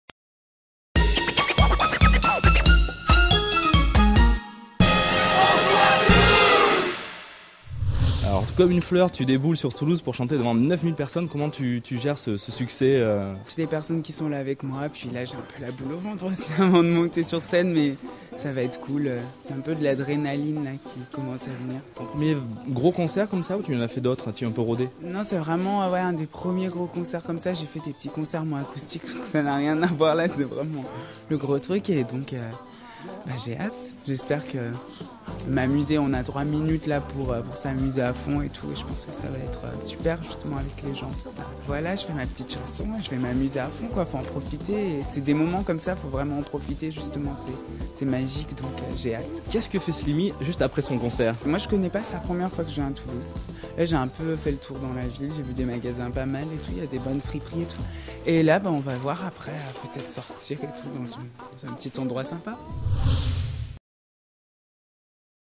Interview Sliimy // Bonsoirtoulouse.com
Interview de Sliimy lors de son passage à Toulouse